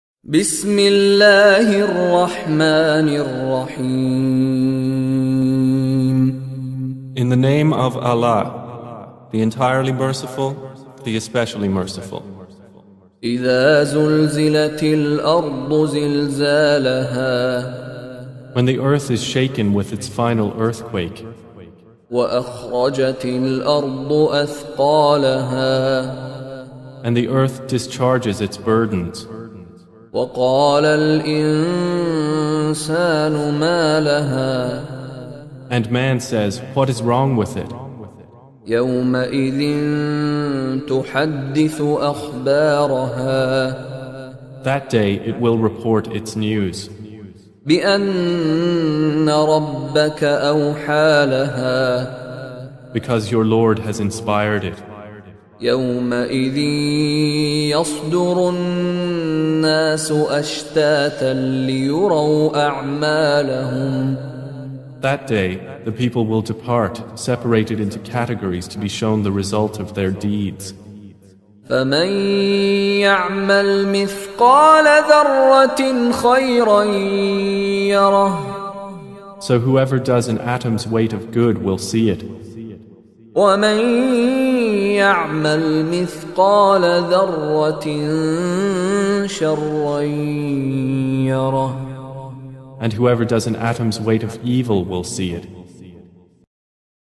Chapter 99 - Surah Az-Zalzala with English Audio Translation
Quran Recitation in Arabic, by Shaykh Mishary bin Rashid Al Afasy, followed by the audio translation of the meaning of each verse in English.